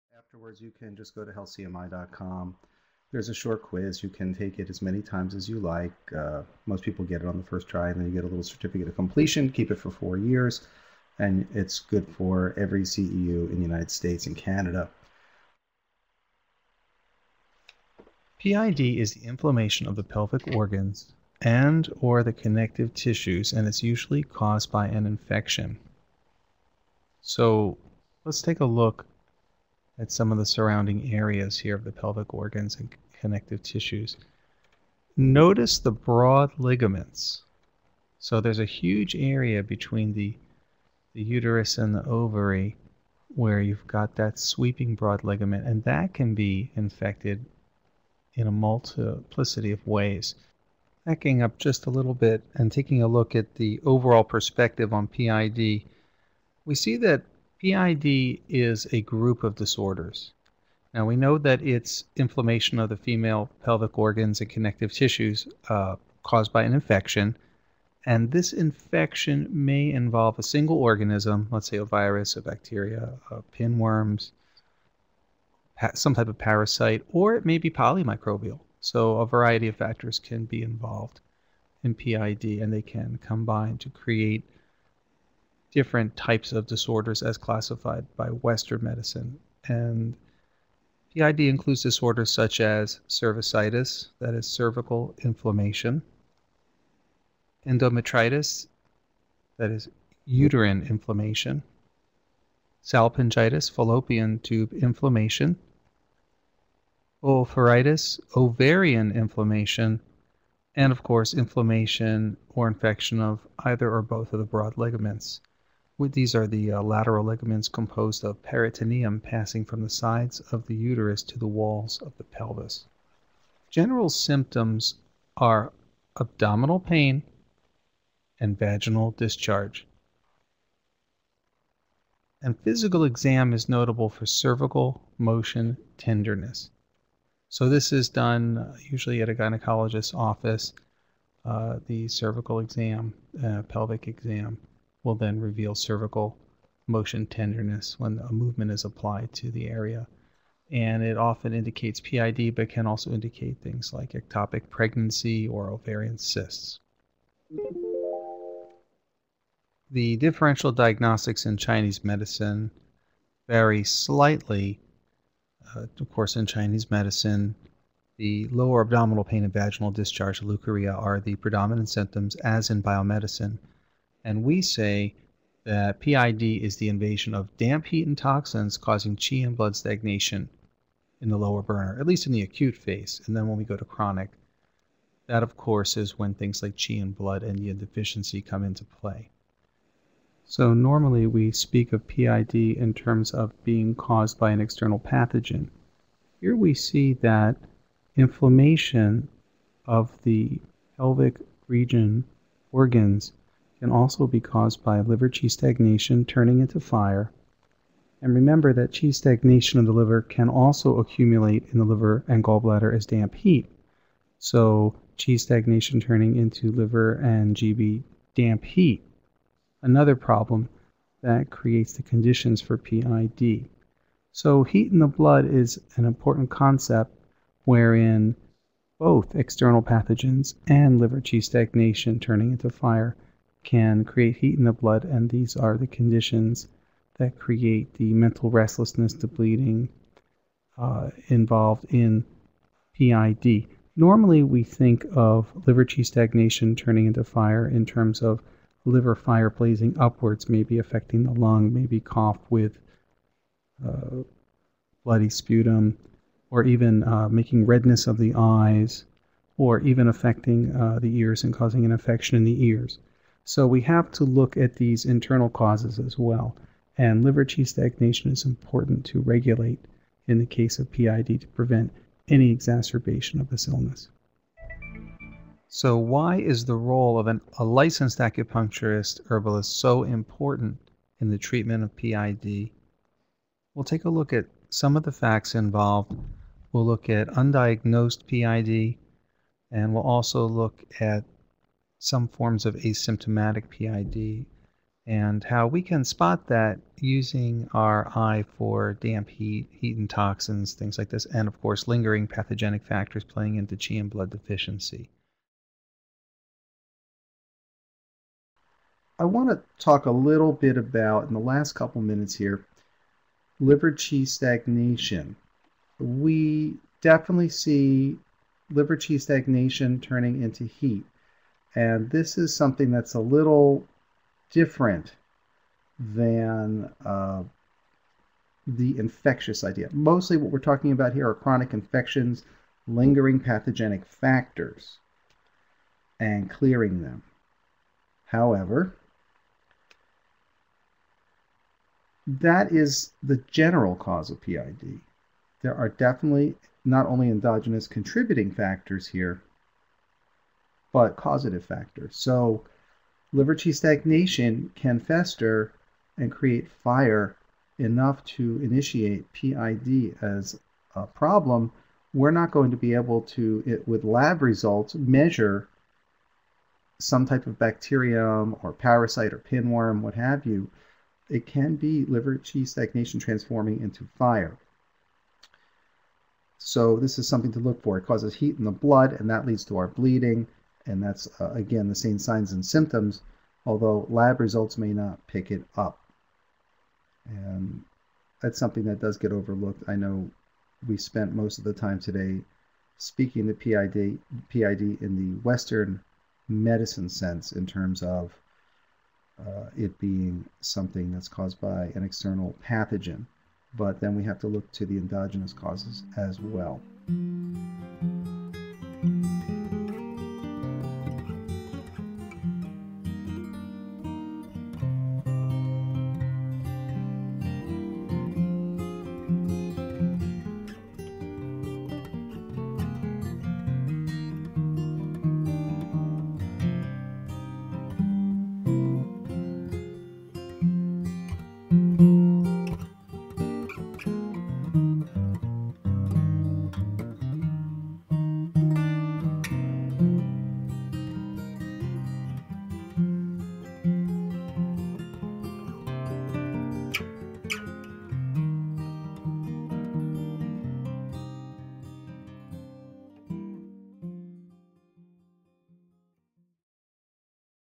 PID and Chinese Medicine | Acupuncture Continuing Education Online
Pelvic inflammatory disease (PID) affects millions of women worldwide every year. This acupuncture continuing education CEU & PDA webinar series covers biomedicine, Chinese medicine theory, herbal medicine and acupuncture treatments.